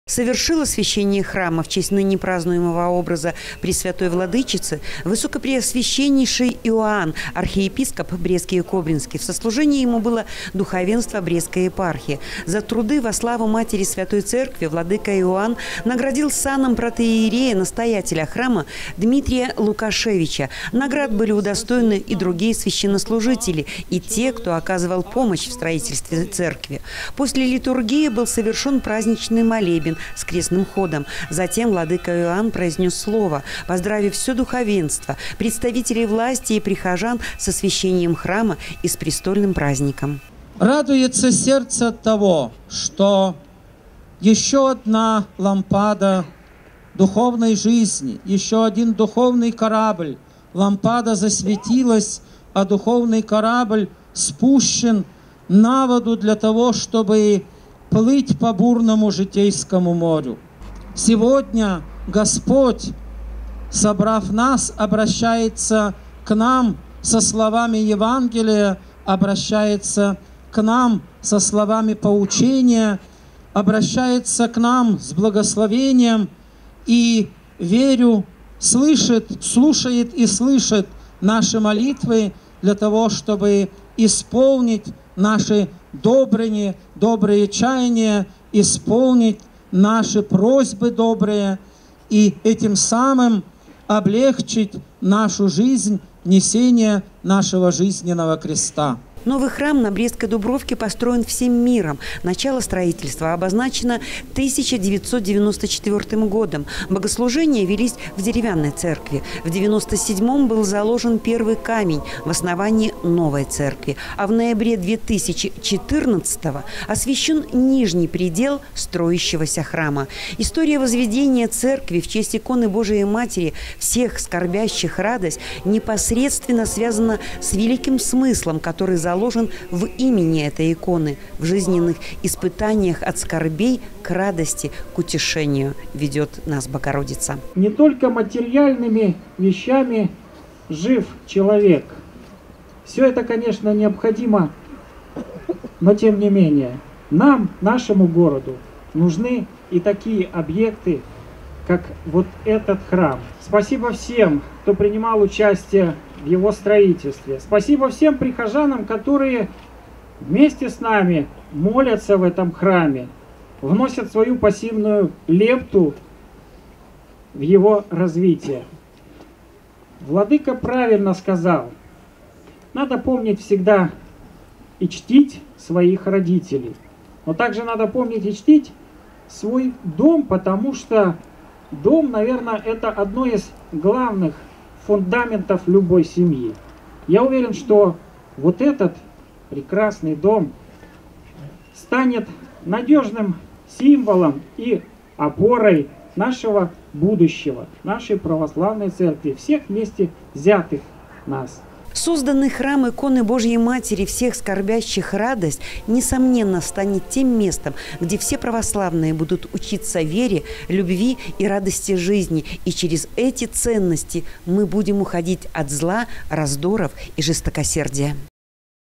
2017-11-07 г. Брест. Освящение церкви. Новости на Буг-ТВ.
Журналист Л. Осмолович Оператор Д. Величкевич.
6 ноября в Бресте на престольный праздник прихода в честь иконы Божией Матери «Всех скорбящих Радость» была освящена новая православная церковь в микрорайоне «Дубровка».